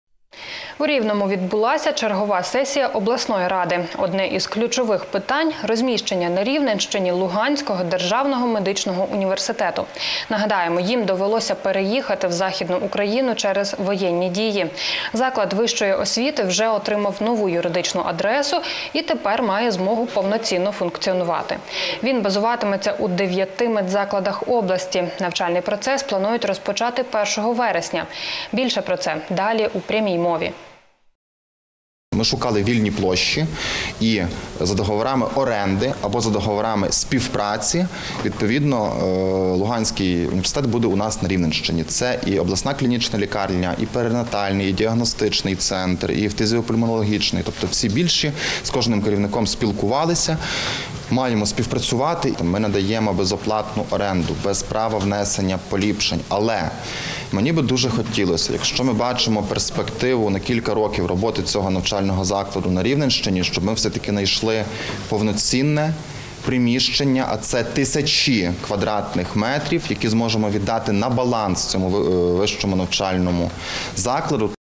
Вперше з початку війни: у Рівному відбулась сесія обласної ради
У Рівному відбулася чергова сесія обласної ради. Одне з ключових питань - розміщення на Рівненщині Луганського державного медичного університету. Нагадаємо, їм довелося переїхати в західну Україну через воєнні дії.

Заклад вищої освіти вже отримав нову юридичну адресу і тепер має змогу повноцінно функціонувати. Він базуватиметься у 9 медзакладах області. Навчальний процес планують розпочати 1 вересня. Більше про це далі у прямій мові.